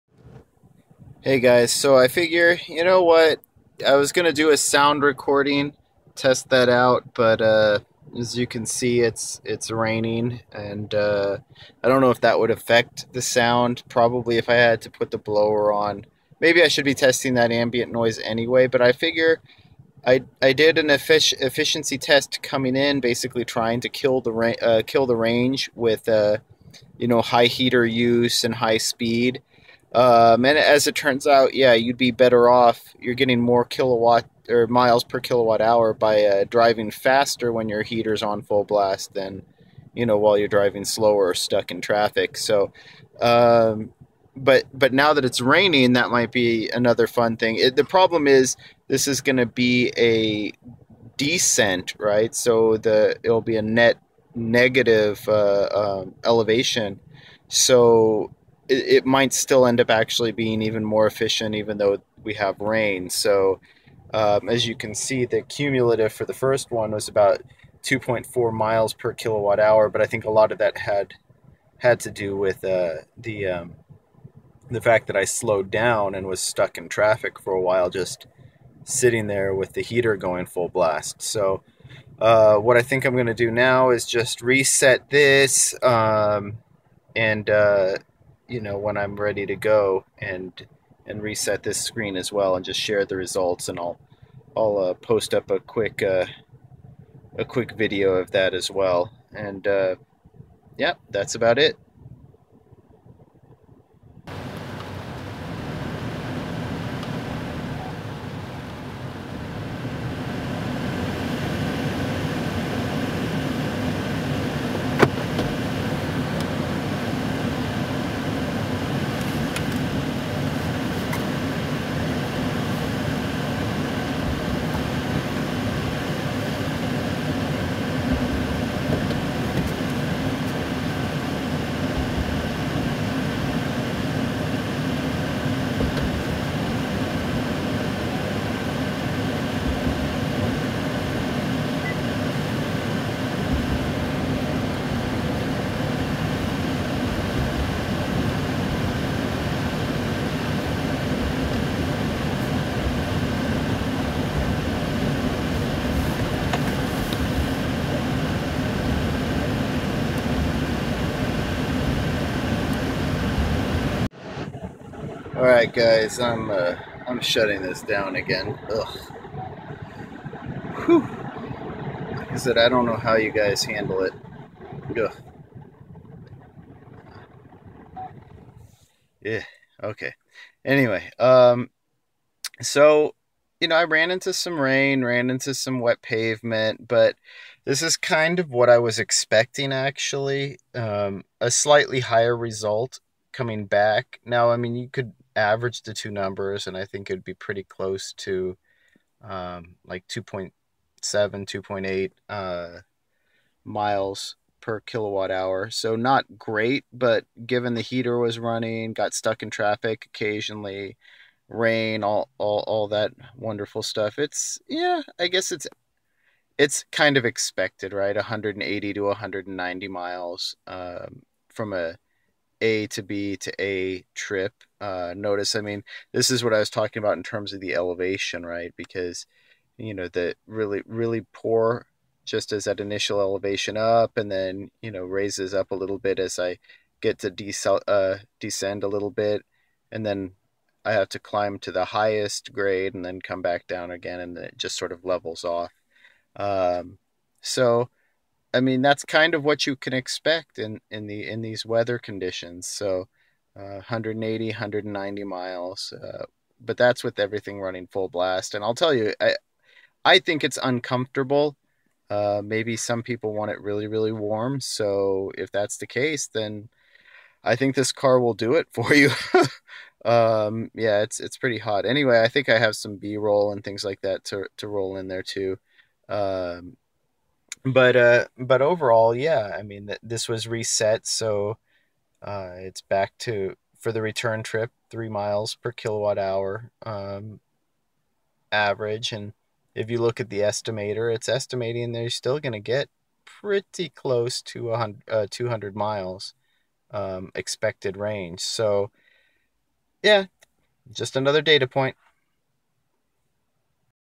Chevy Bolt EV Low Efficiency Driving Return Trip
Inefficient return trip. Heater on max with rain. This is a bit more efficient because of the decent, and it looks like an average of 3 mi/kWh could be expected for A to B to A trips in these conditions.

Again, I only got a short run of b-roll. Working on the camera.